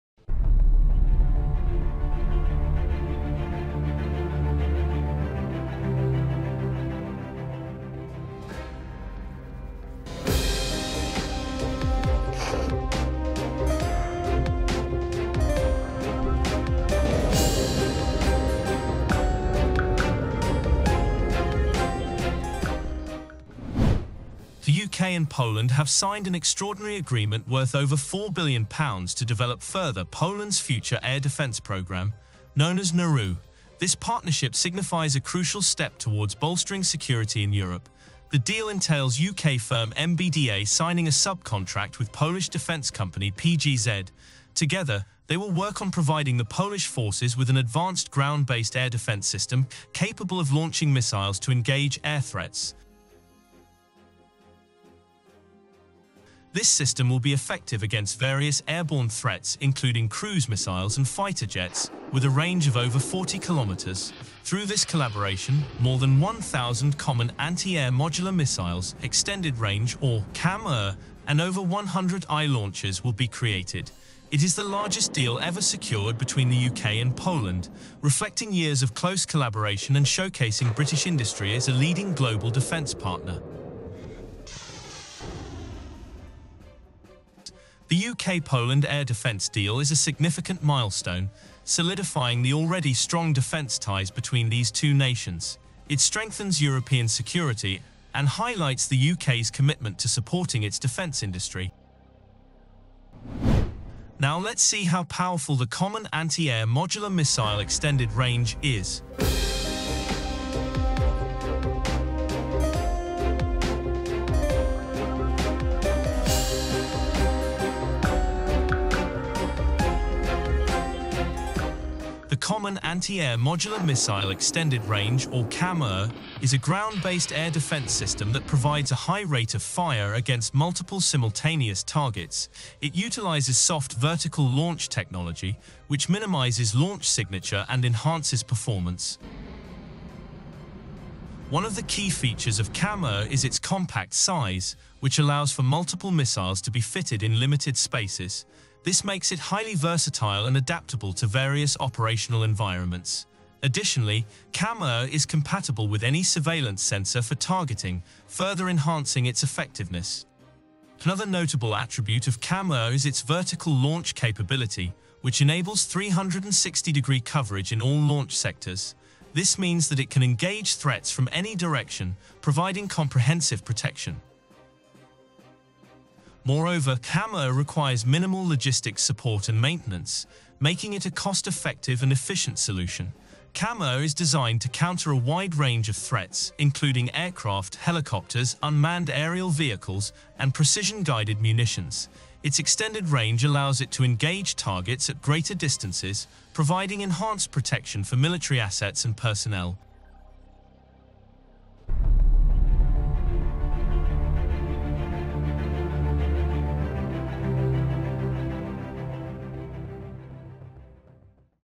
£4 billion UK-Poland Air Defence Deal: How powerful the CAMM-ER air defence systems is?
The UK and Poland have signed an extraordinary agreement worth over £4 billion to develop further Poland's future air defense program, known as Narew. This partnership signifies a crucial step towards bolstering security in Europe. Through this collaboration, more than 1,000 Common Anti-Air Modular Missiles - Extended Range (CAMM-ER) and over 100 iLaunchers will be created.